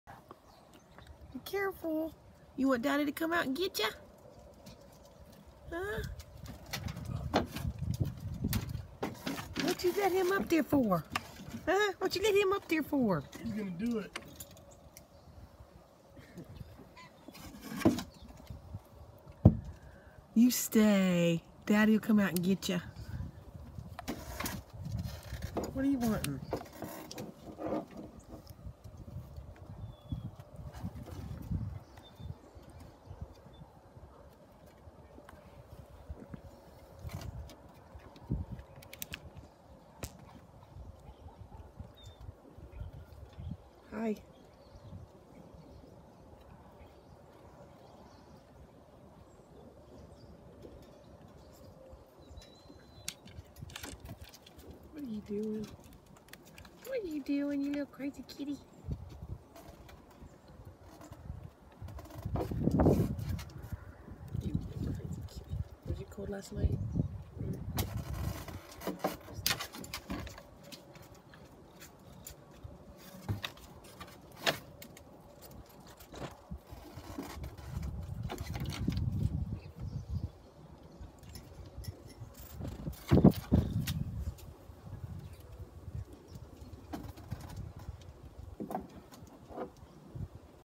Axle our great Pyrenees pup caught on trampoline again all by himself, big boy
Addie & Jinger with Axle who’s got himself on trampoline  end of Dec.2020